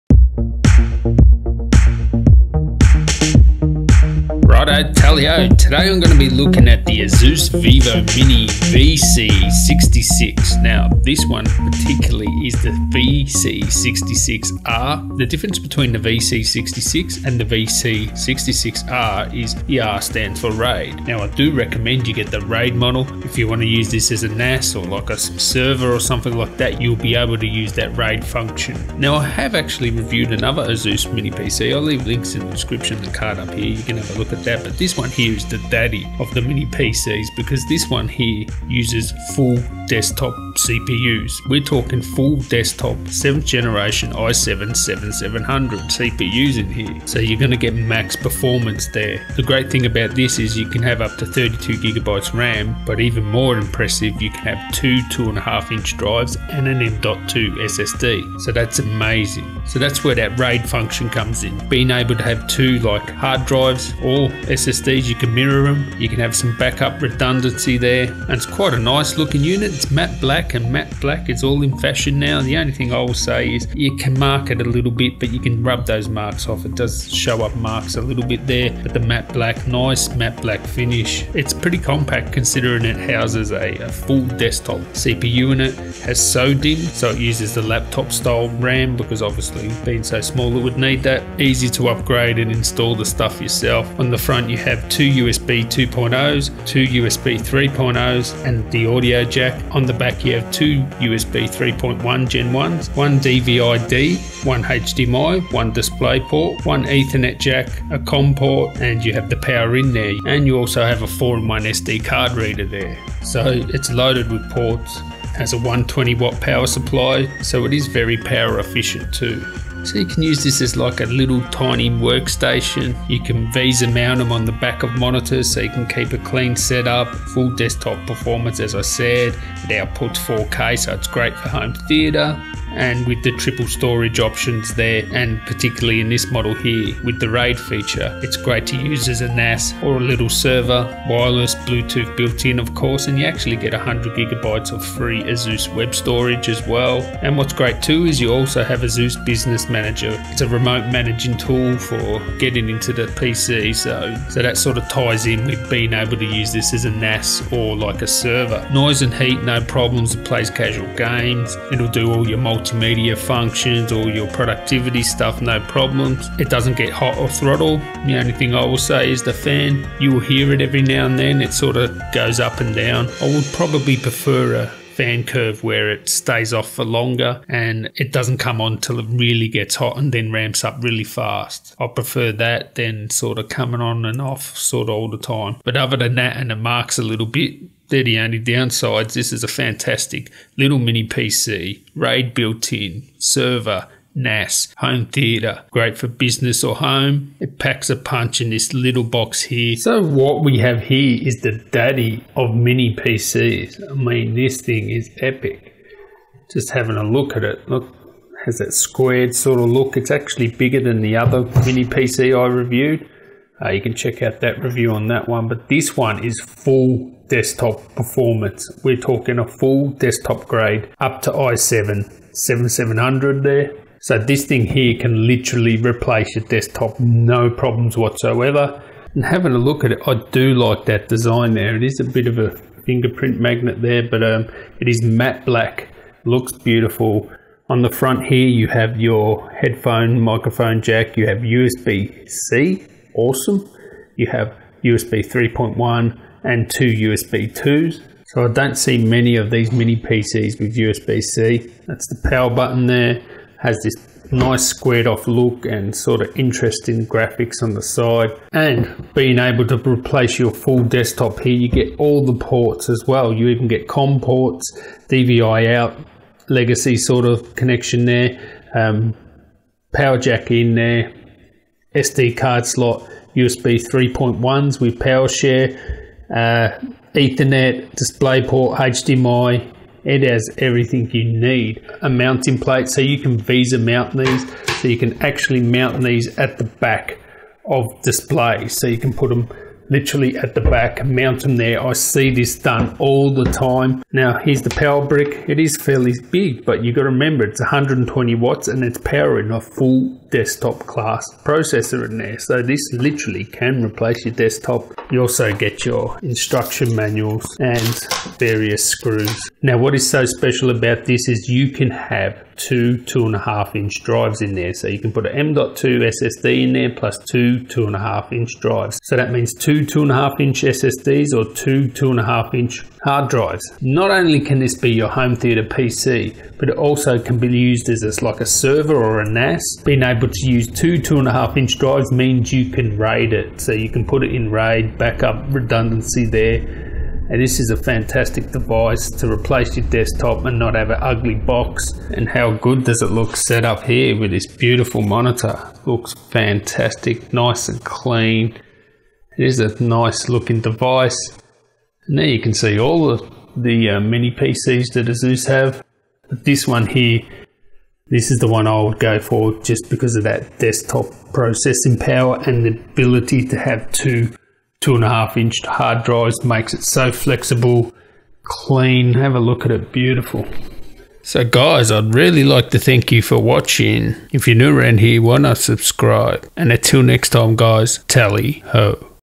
ASUS VivoMini VC66R Mini PC REVIEW Great for HTPC NAS Server Workstation
ASUS VivoMini VC66R Mini PC REVIEW Great for HTPC NAS Server WorkstationHigh-performance mini PC with an Intel® Core™ i7, i5 , i3 processor, Windows 10 Home, DDR4 RAM, 4K UHD support, RAID support and triple storage up to one M.2 SSD and two SSD/HDD, and 802.11ac Wi-Fi.